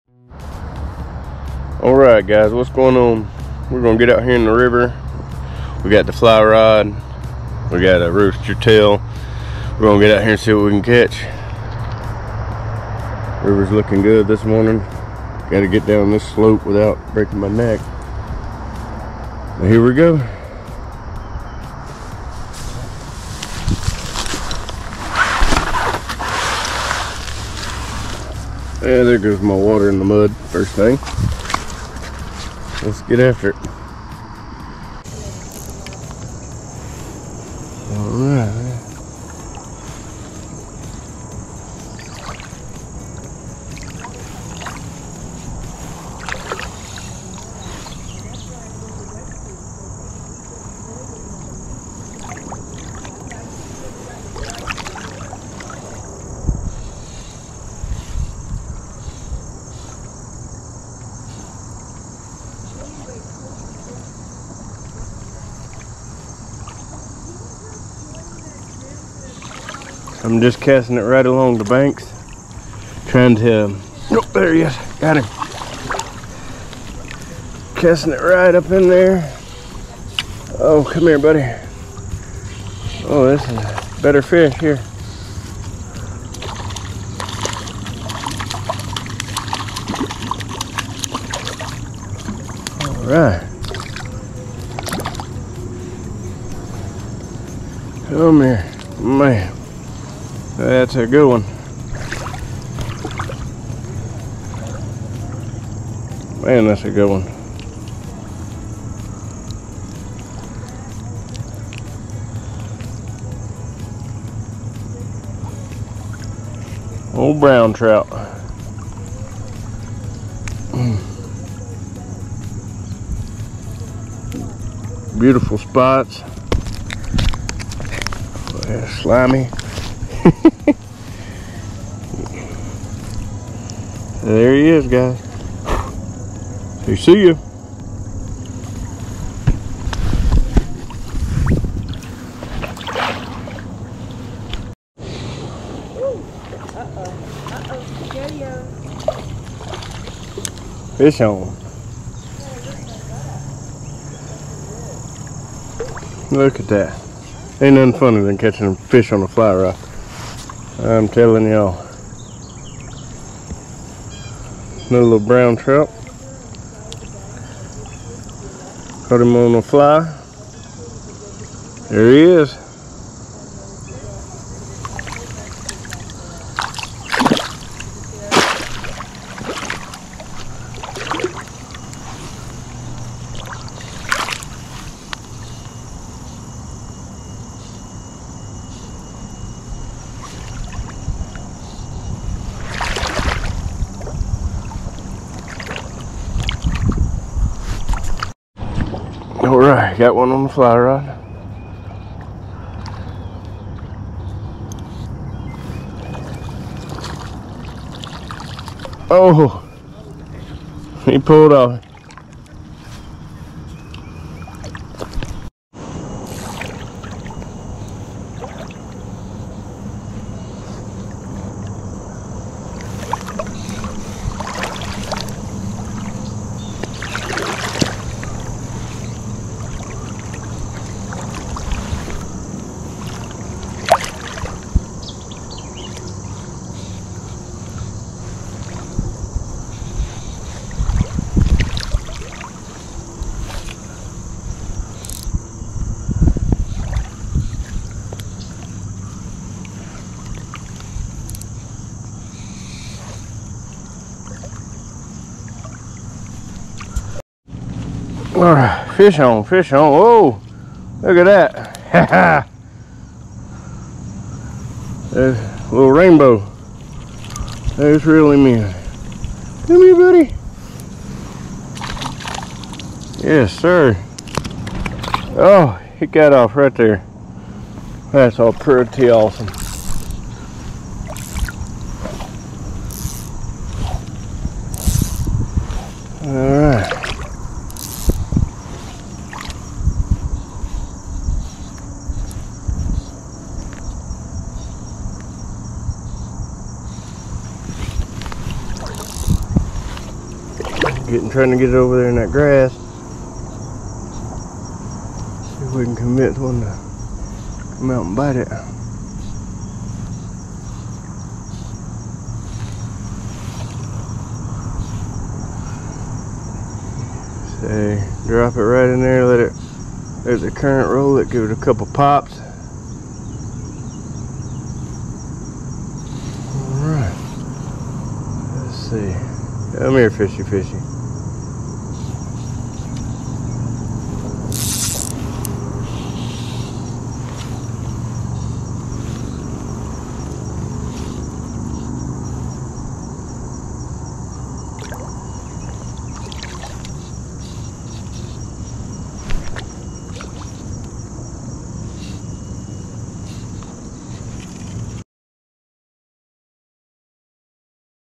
Fly Fishing in north GA… When this happened!!!!!
Today we are fly fishing in north ga.
We are fishing the Toccoa River above lake blue ridge.